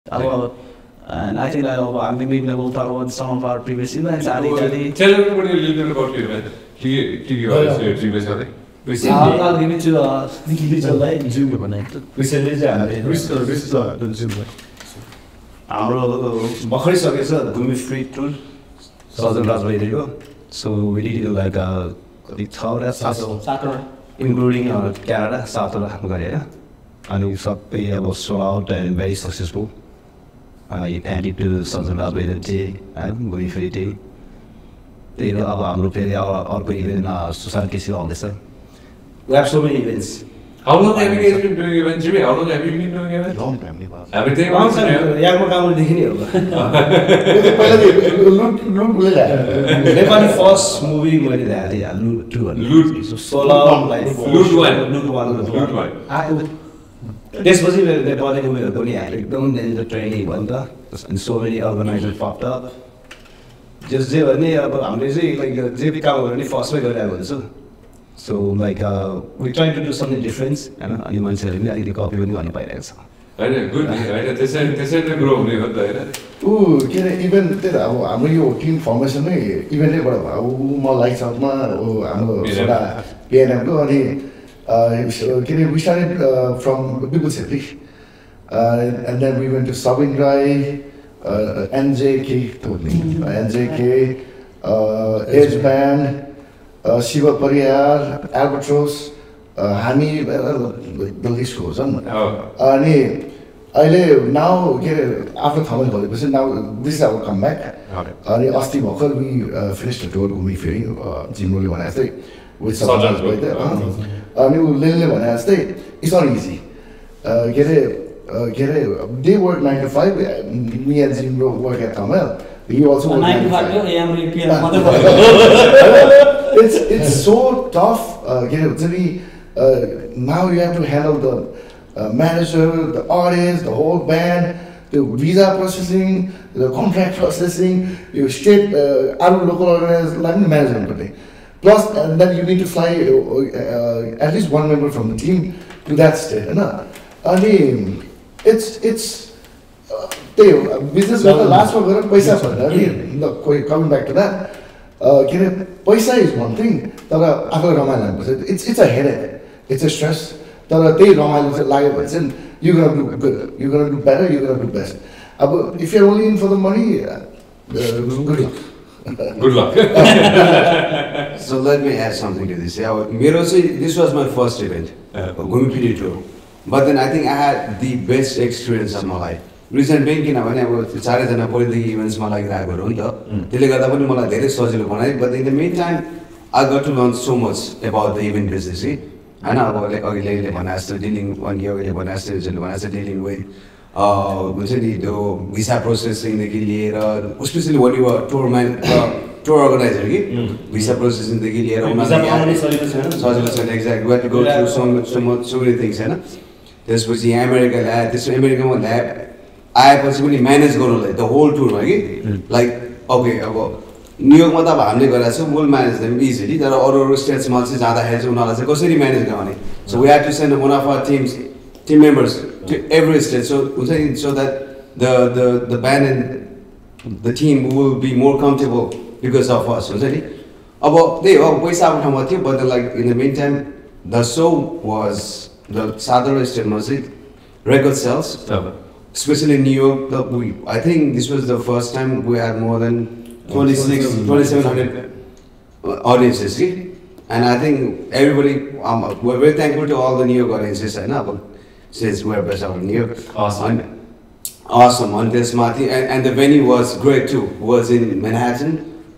Hosting Unforgettable Events: Navigating The Highs & Lows! | Jimmy Gurung
"Expanding Horizons: "On Air" Podcast Goes Global, Unveiling a Dynamic Journey to the US".

Meet the team Thamel NYC, Nijjwol Lamsal, a senior partner engineer at Cloudera and the brains behind Thamel NYC. Lelin Kandel, training manager at Imperial Advance, is also a Partner at Thamel NYC . And that's not all – we've got the powerhouse trio of Roshan Gauchan, Jimmy Gurung, and Yogendra Limbu, all working their magic at Thamel NYC.